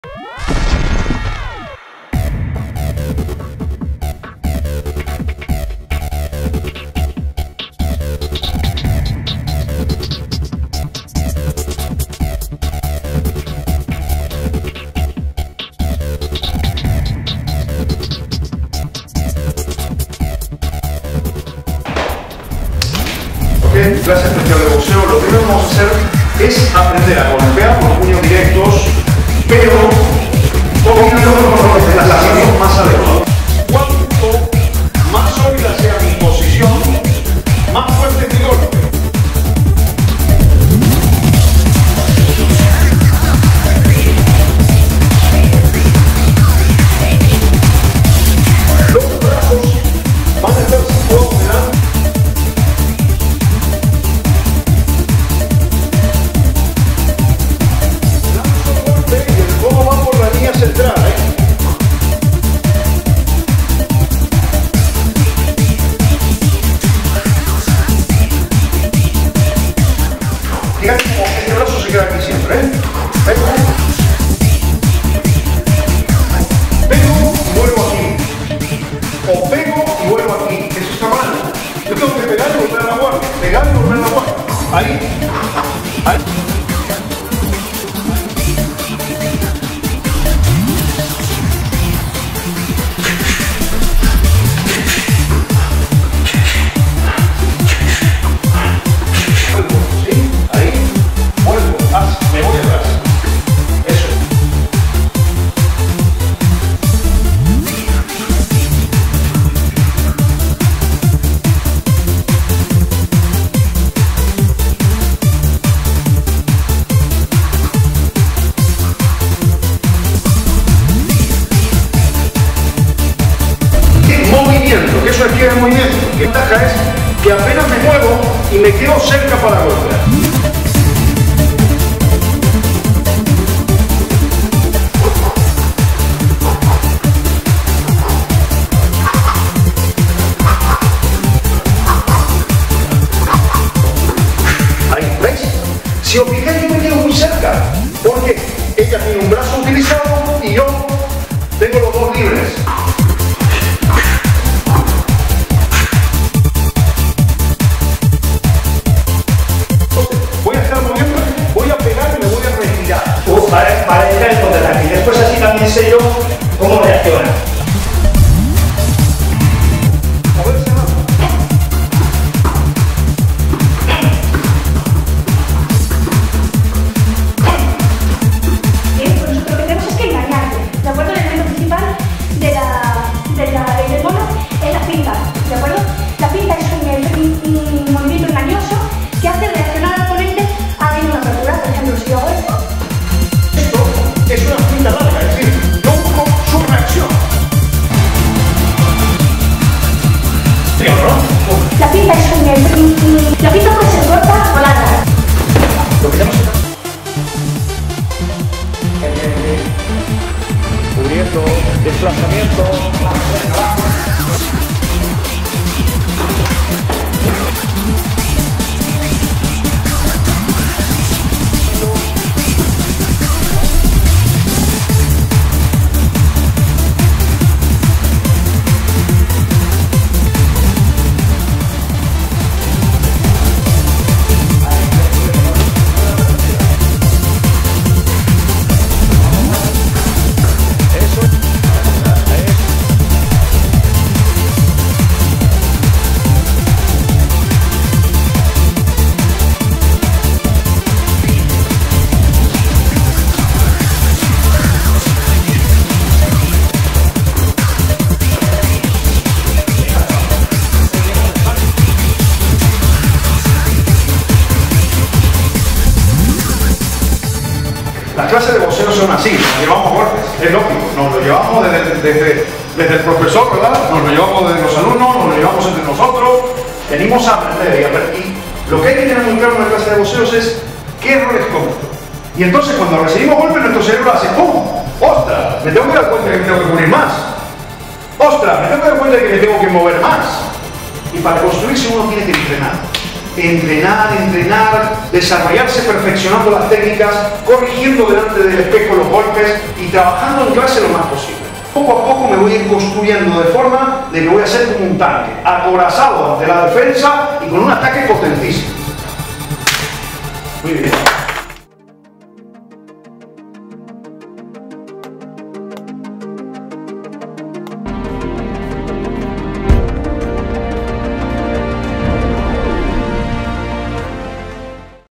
CURSO DE BOXEO TAIKIC - POR YEE SEIL - DEMO
CURSO COMPLETO ESPECIAL DE BOXEO Y TÁCTICAS TAIKIC dirigido por el maestro Yee Seil. Posiciones, guardias, principales golpes, esquivas, desplazamientos, fintas, ejercicios de entrenamiento y combates,... . Este video es el trailer o demo del curso si lo quieres ver completo ve a este link: